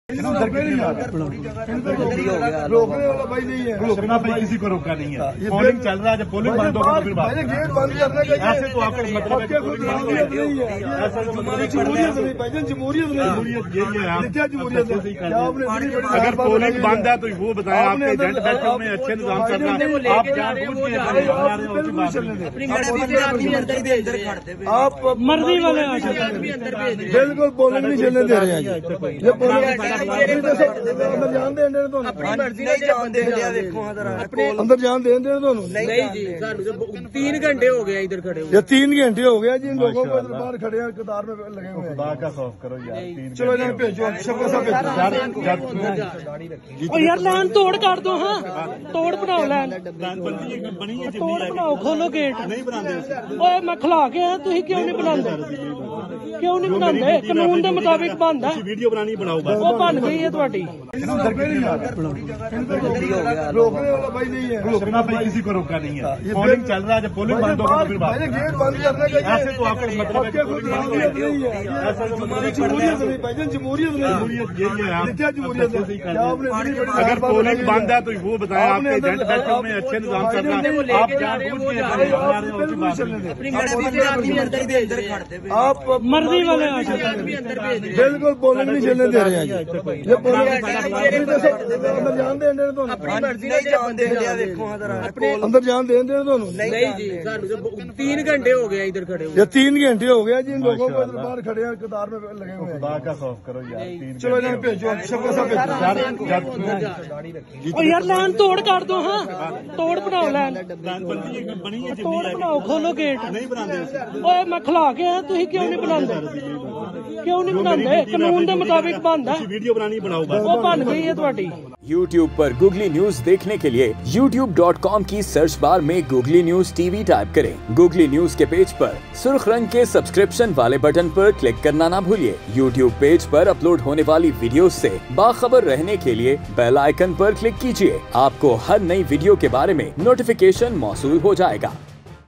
PMLN Kai Voters Ko Polling Station Mein Dakhlay Sai Roknay Kai Manazir | Googly News TV
PMLN Kai Voters Ko Polling Station Mein Dakhlay Sai Roknay Kai Manazir